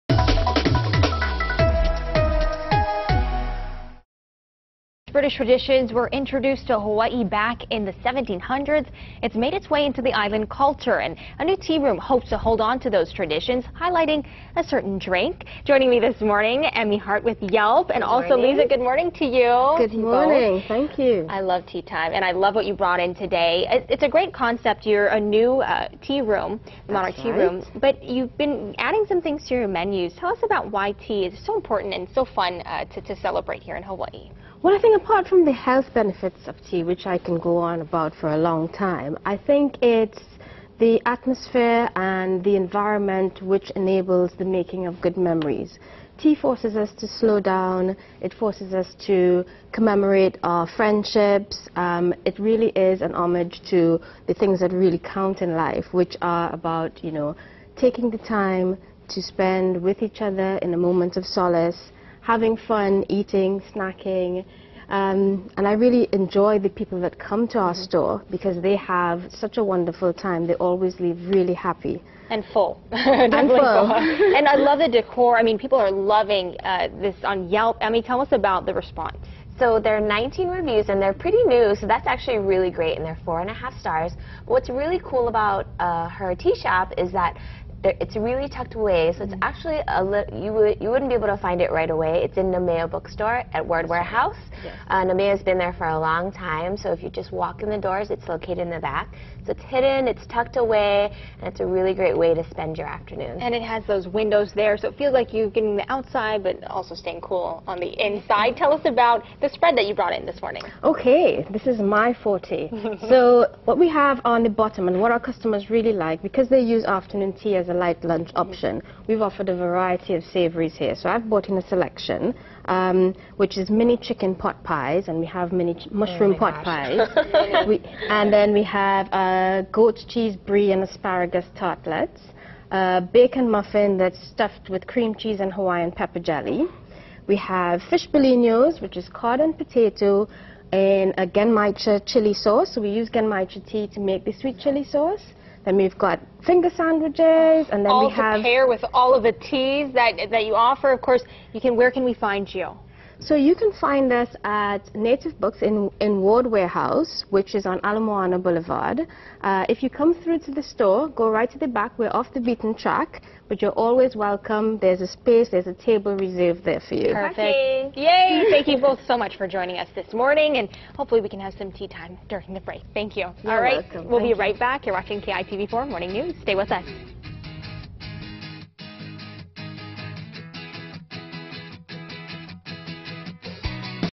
Low-key High Tea at the Monarch Tea Room
Yelp introduces you to the Hawaiian tradition of English tea with Liza Yunis from the Monarch Tea Room.